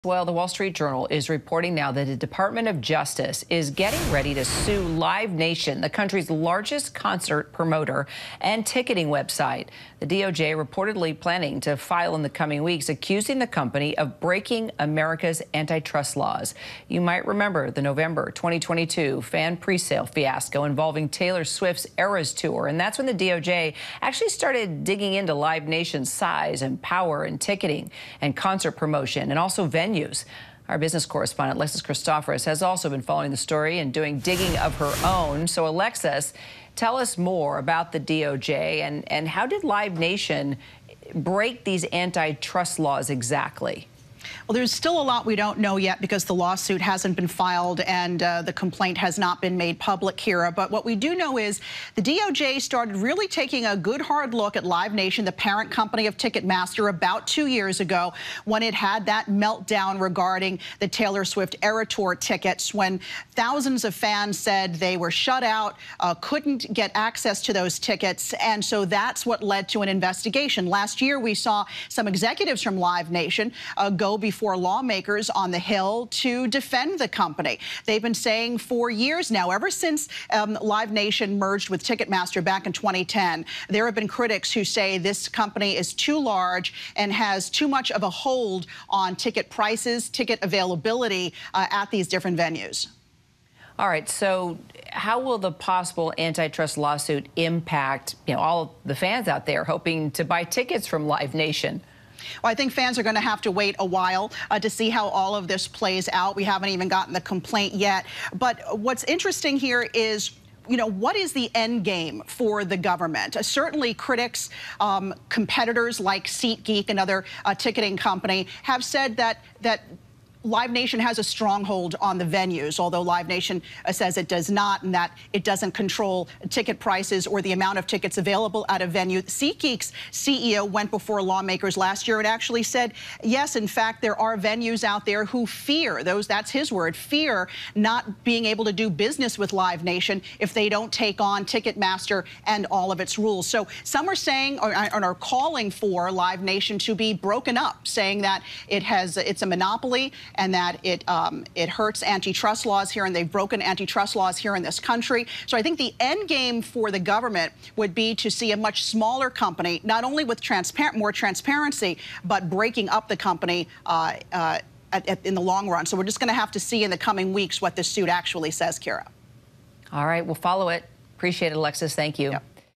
Justice Department to file antitrust suit against Live Nation
The Department of Justice is gearing up to sue Live Nation, the country's large ticketing website. ABC News' Alexis Christoforous reports.